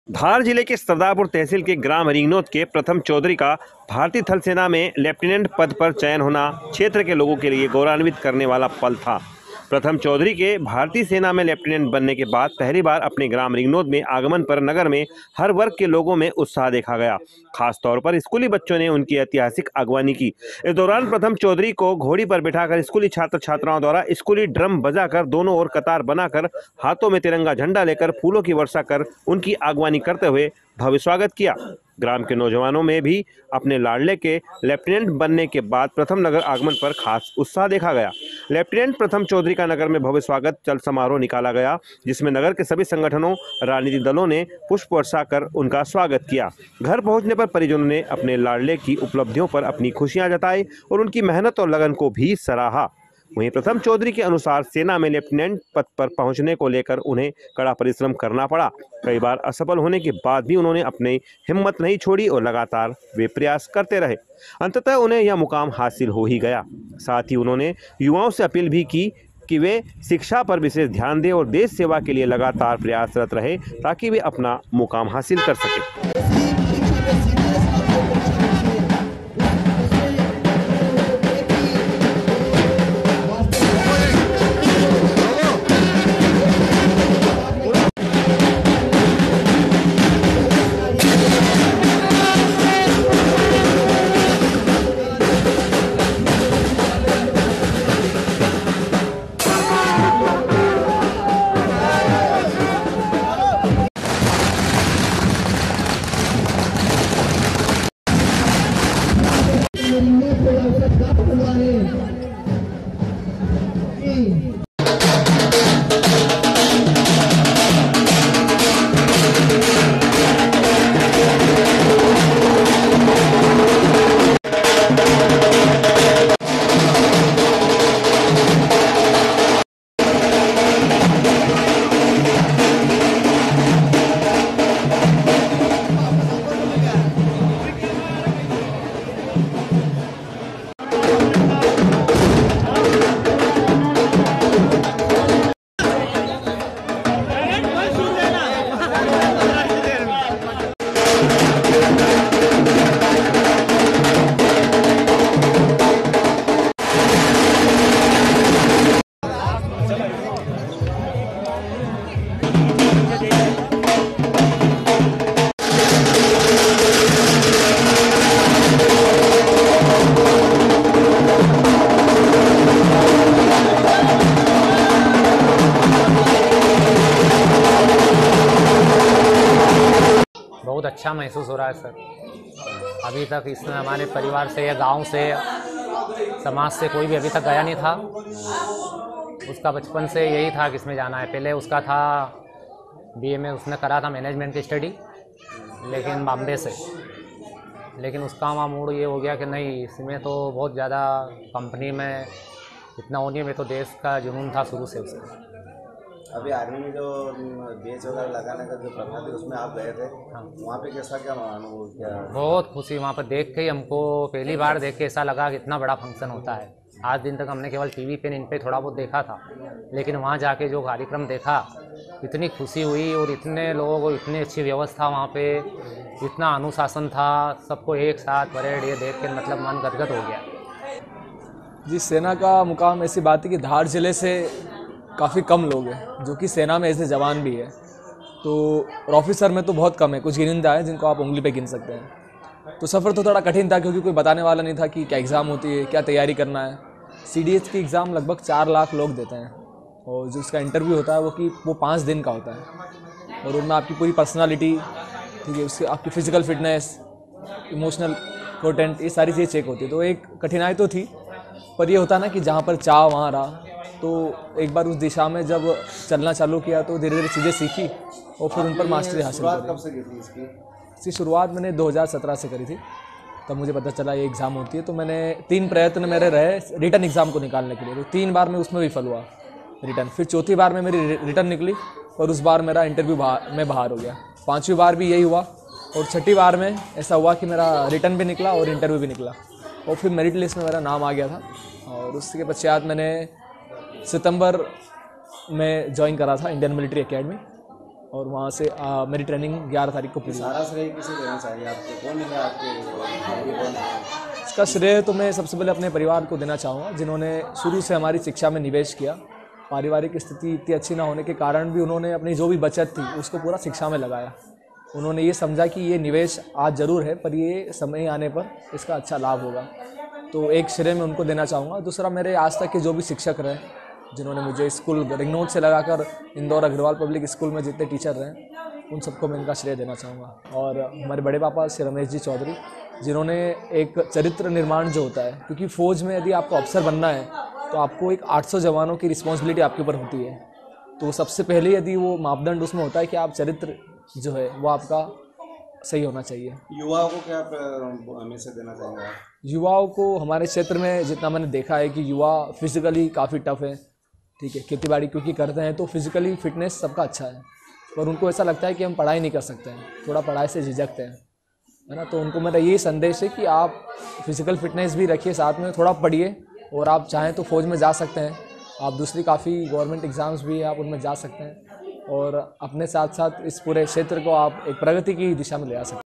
सेना में लेफ्टिनेंट बन आने पर छात्रों ने घोड़े पर बैठा तिरंगे ले बैंड बजा की आगवानी, ऐतिहासिक स्वागत
धार  -  सेना में लेफ्टिनेंट बन प्रथम आगमन पर रिंगनोद में उत्साही छात्र छात्राओं ने घोड़े पर बैठा तिरंगा ध्वज हाथ में ले बैंड के साथ की आगवानी , ग्रामीणों ने बेटे का चल समारोह निकाल किया ऐतिहासिक स्वागत , पूरी खबर देखें लोकप्रिय धार न्यूज़ चैनल पर